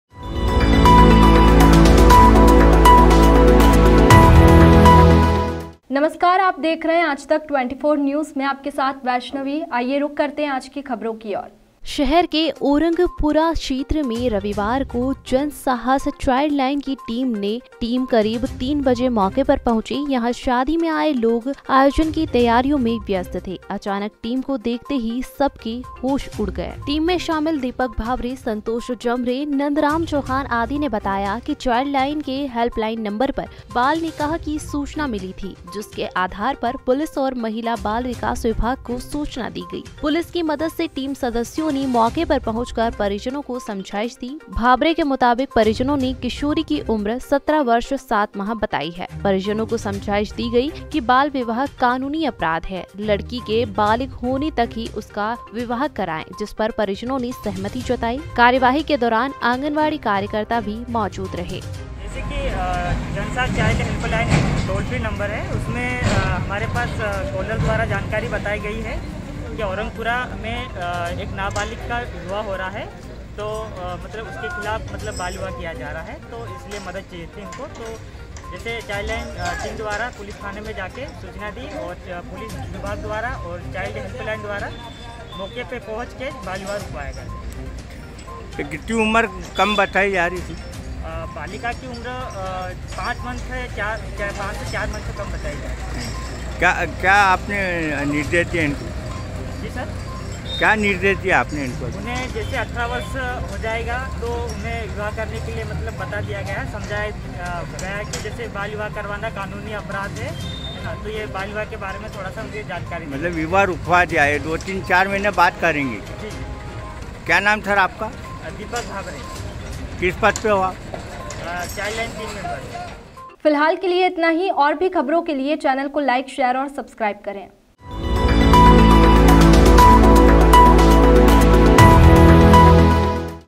चाईल्ड लाईन ने रुकवाया बाल निकाह। शहर के औरंगपुरा क्षेत्र में रविवार को जनसाहस चाइल्ड लाइन की टी
चाईल्ड लाईन ने रुकवाया बाल निकाह

खरगोन। शहर के औरंगपुरा क्षेत्र में रविवार को जनसाहस चाइल्ड लाइन की टीम ने पुलिस की मदद से एक किशोरी का निकाह रुकवाया। टीम करीब 3 बजे मौके पर पहुंची। यहां शादी में आये लोग आयोजन की तैयारियोंं में व्यस्त थे, अचानक टीम को देखते ही सबके होश उड़ गए।  टीम में शामिल दीपक भाभरे, संतोष जमरे, नंदराम चौहान आदि ने बताया कि चाईल्ड लाईन के हेल्पलाईन नंबर पर बाल निकाह की सूचना मिली थी, जिसके आधार पर पुलिस और महिला बाल विकास विभाग को सूचना दी गई। पुलिस की मदद से टीम सदस्यों ने मौके पर पहुंचकर परिजनों को समझाईश दी। भाभरे के मुताबिक परिजनों से किशोरी की उम्र    17 वर्ष 7 माह बताई गई। परिजनों को समझाईश दी कि बाल विवाह कानूनी अपराध है, लड़की के बालिग होने पर ही उसका विवाह करांए, जिस पर परिजनों ने सहमति जताई। कार्रवाई के दौरान आंगनवाड़ी कार्यकर्ता भी मौजूद रहे।
बाईट। दिपक ठाकरे
चाईल्ड लाईन